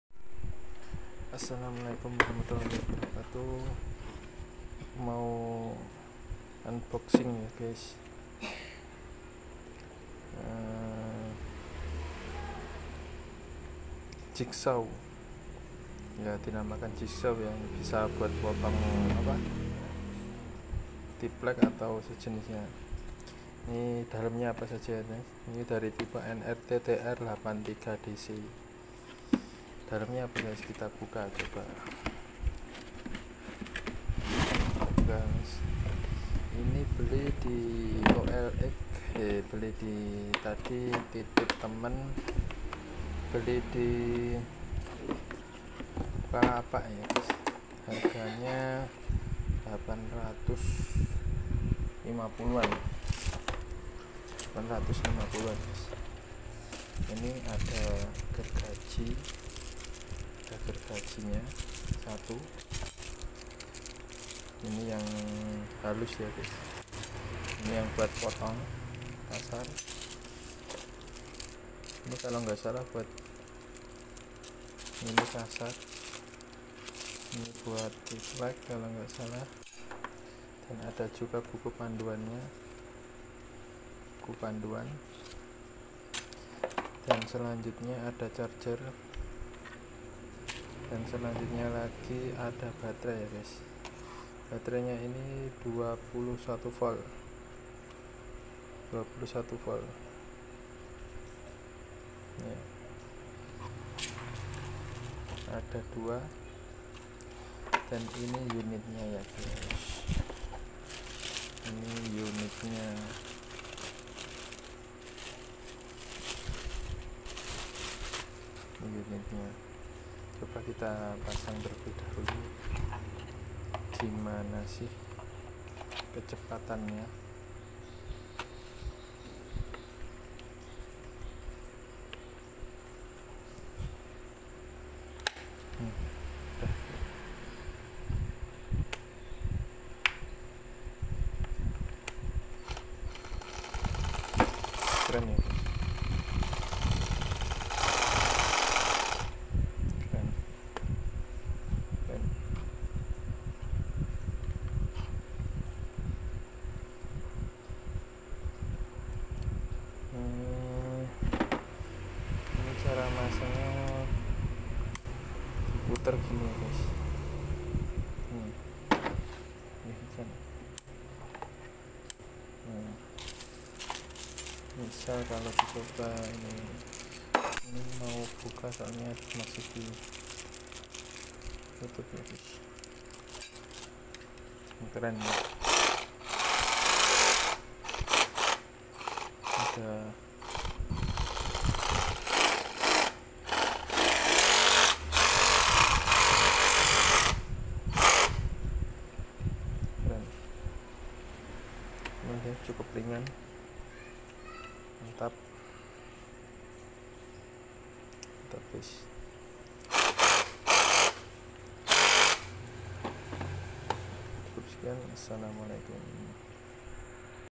Unboxing gergaji batrai saw NRT TR-83DC
Cordless Recipro Saw NRT PRO TR83DC

Merk : NRT PRO
Seri : TR 83 DC
Baterai : 21V
Kecepatan : 0-2700spm
Kapasitas Potong :
- Kayu : 250mm
- Besi : 20mm
- Pipa : 130mm

isi paket :
1x unit mesin gergaji
2x batrei 21v 2.0 Ah
1x charger
3x mata
1x kotak penyimpanan